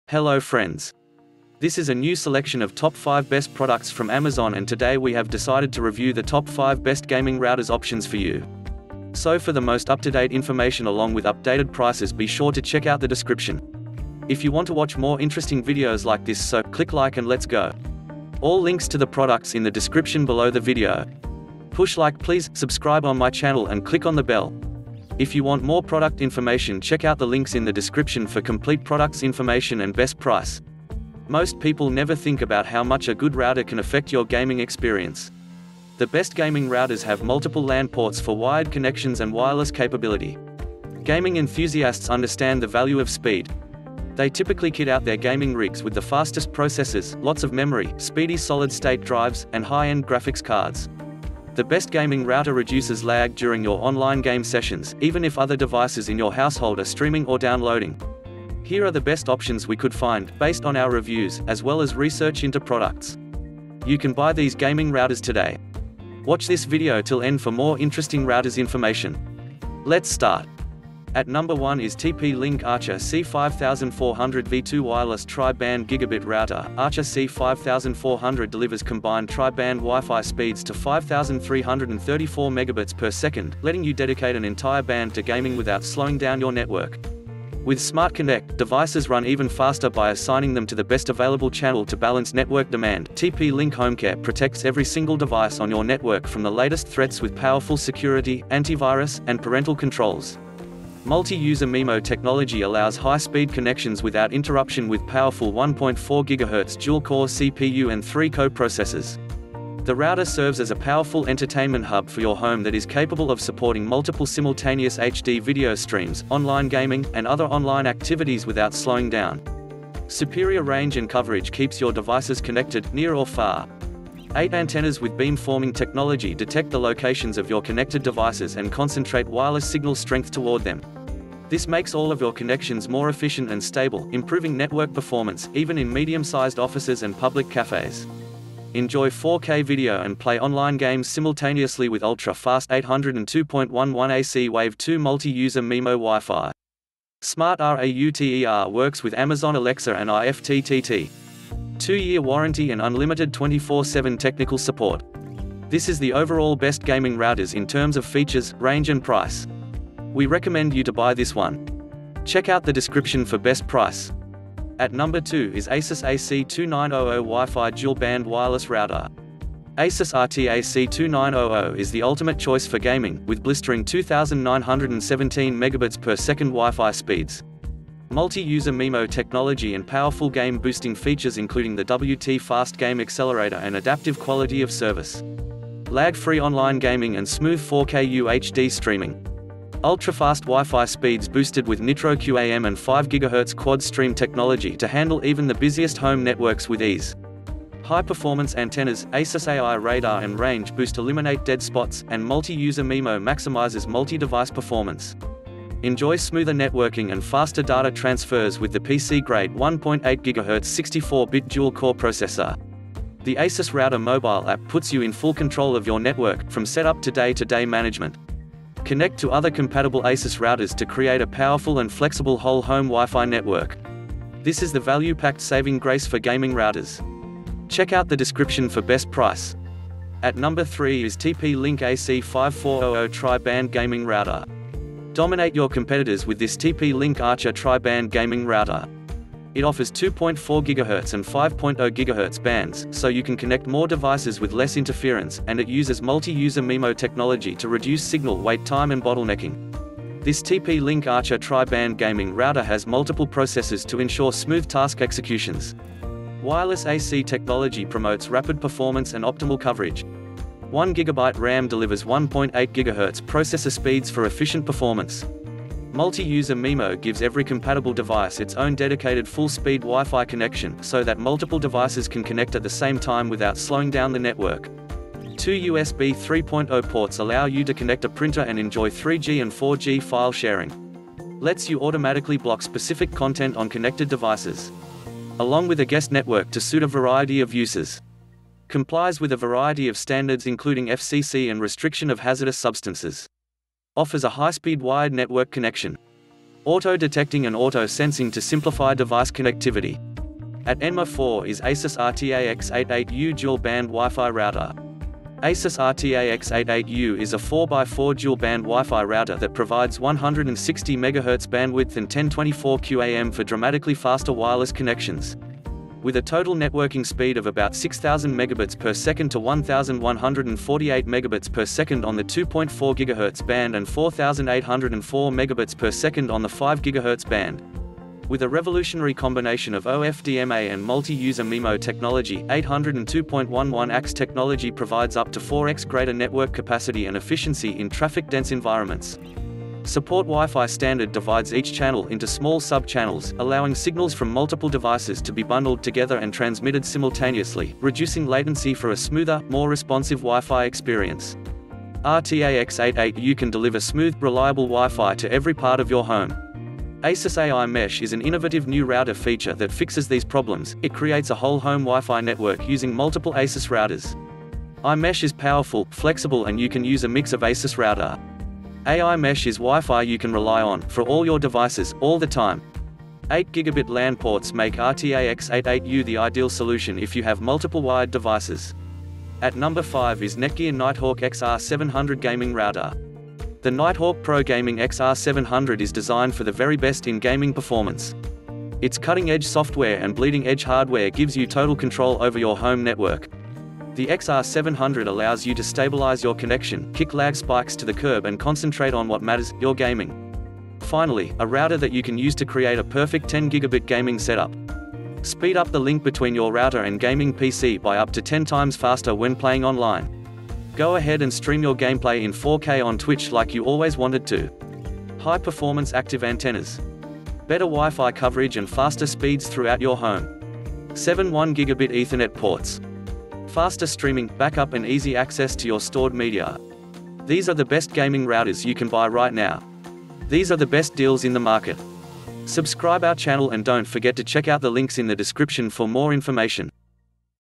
Best Gaming Router in 2021 [Top 5 Picks For Fast Internet Speed]
1. TP-Link Archer C5400 v2 Wireless Tri-Band Gigabit Rauter
2. ASUS AC2900 Wi-Fi Dual-band Gigabit Wireless Rauter
3. TP-Link AC5400 Tri Band Gaming Rauter
4. pASUS RT-AX88U Dual-Band WiFi Rauter
5. Netgear Nighthawk XR700